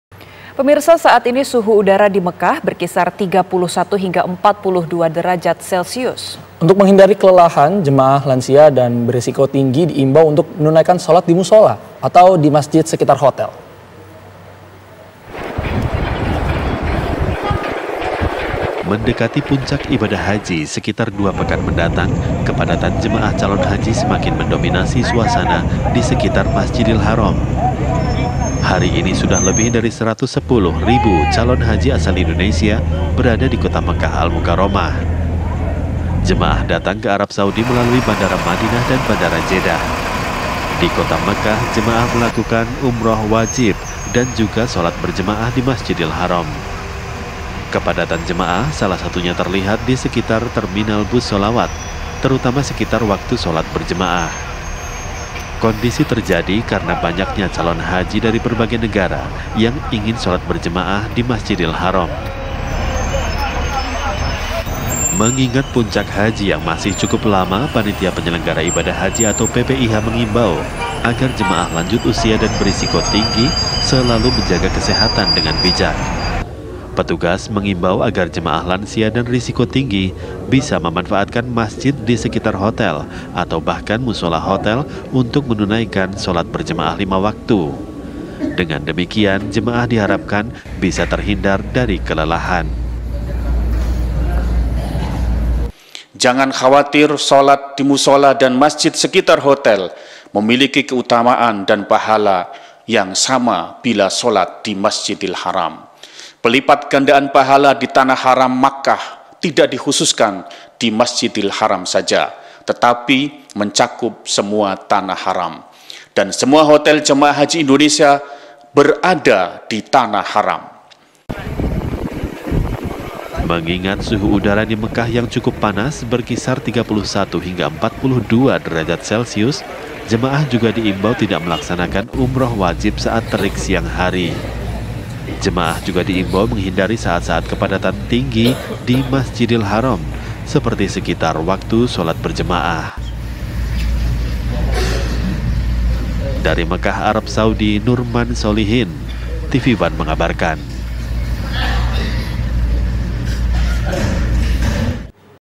Masjidil Haram Mulai Dipadati Jemaah Calon Haji dari Berbagai Negara | Berita Islam
Berita Islam, religiOne - Masjidil Haram mulai dipadati jemaah haji

Masjidil Haram mulai dipadati umat muslim dari berbagai negara di dunia, sedangkan jemaah calon haji Indonesia menuju Mekah secara bertahap mulai 1 Juni 2023 malam.

Sementara itu, Sebanyak 450 armada bus Shalawat dengan jumlah petugas sebanyak 230 orang petugas disiapkan untuk memfasilitasi mobilitas serta aktifitas jemaah haji Indonesia selama beribadah 24 jam penuh di Kota Makkah, Arab Saudi.